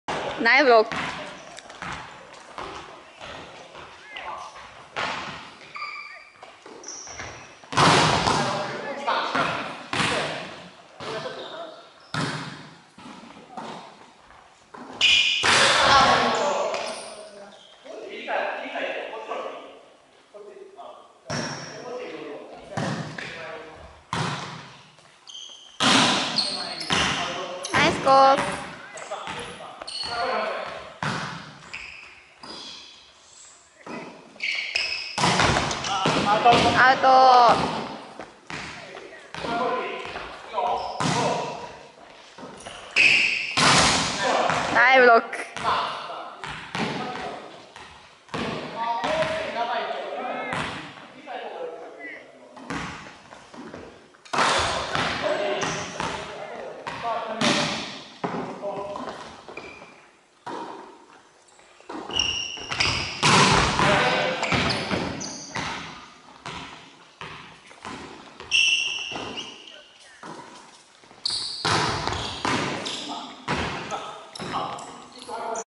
7月21日 トス＆アタック練習 2
バレーボールサークル　No.8　の練習風景です。プロのブロック付き。